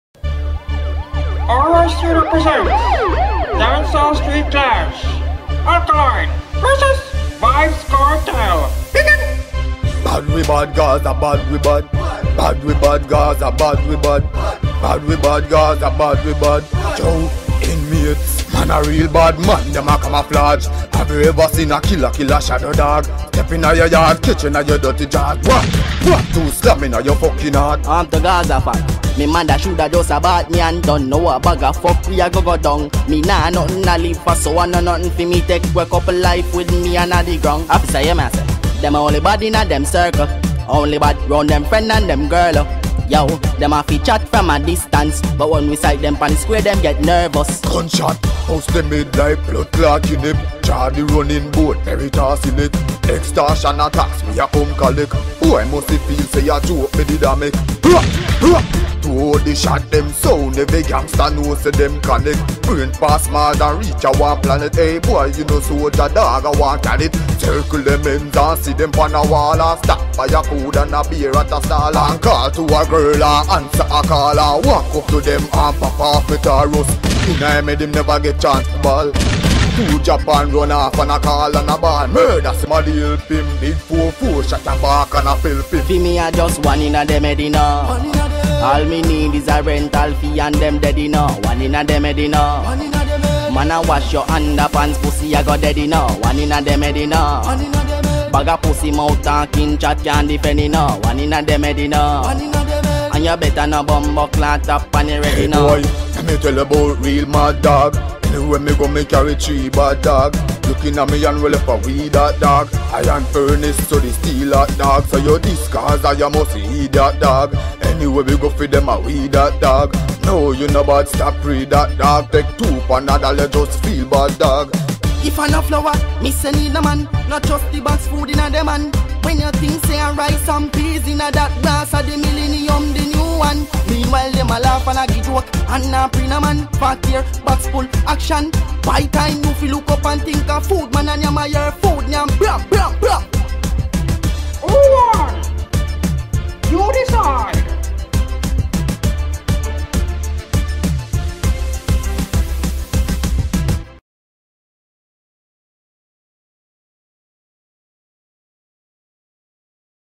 Alkaline Vs Vybz Kartel. [Dancehall Street Clash] 2016 - New 2017
Vybz Kartel Vs Alkaline. [ Dancehall Street Clash]
Created By Zeej Sterling.
Voices By Zeej Sterling,Don Terro and Vince WIlson
Music Composition by Zeej Sterling.